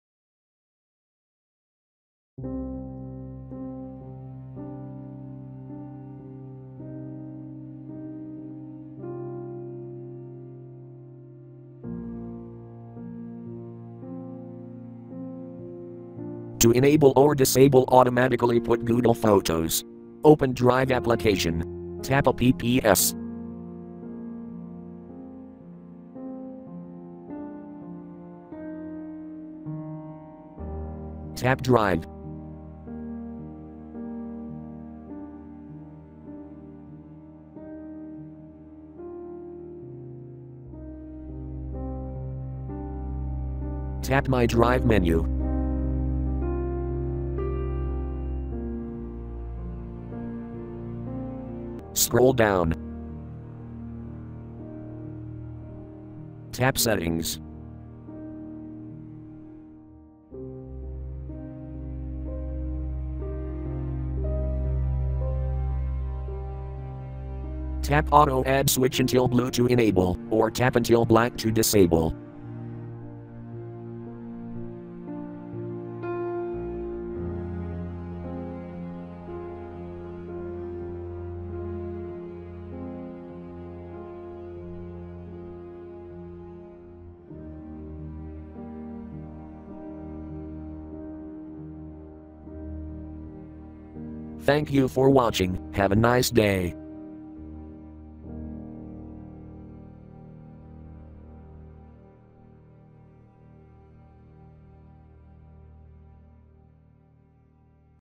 Google Drive for Android : How to Enable or Disable Automatically put google Photos
This video show How to Enable or Disable Automatically put google Photos on Google Drive.  In this tutorial I use Samsung Galaxy S6 Edge SM-G925F International version with Android 5.1 (Lollipop).